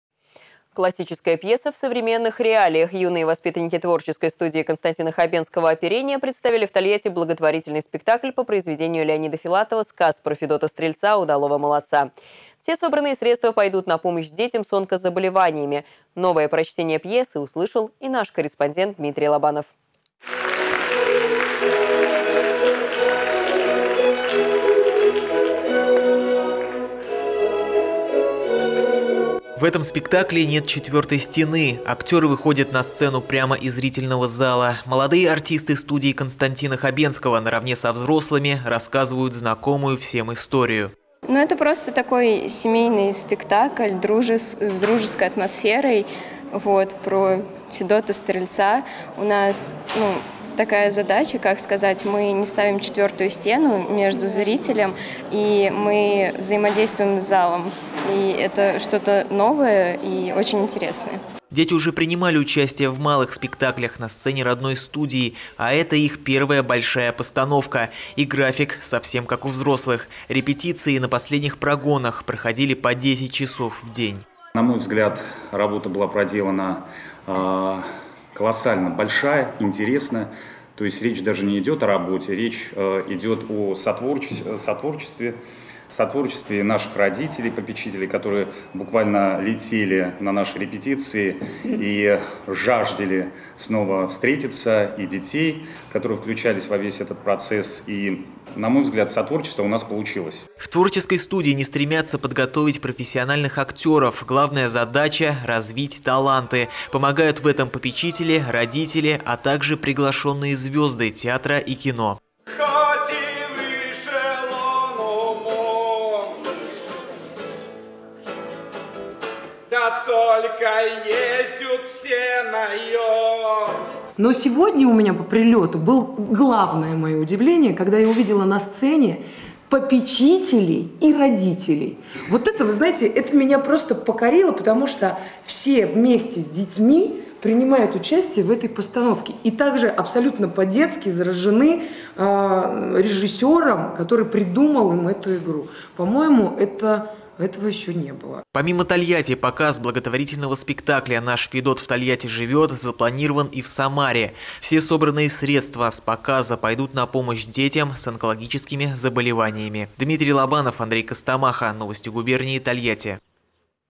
Воспитанники студии “Оперение” представили в Тольятти благотворительный спектакль
Классическая пьеса в современных реалиях. Юные воспитанники творческой студии Константина Хабенского «Оперение» представили в Тольятти благотворительный спектакль по произведению Леонида Филатова «Сказ про Федота-Стрельца, удалого молодца». Все собранные средства пойдут на помощь детям с онкозаболеваниями. Новое прочтение пьесы услышал и наш корреспондент Дмитрий Лобанов.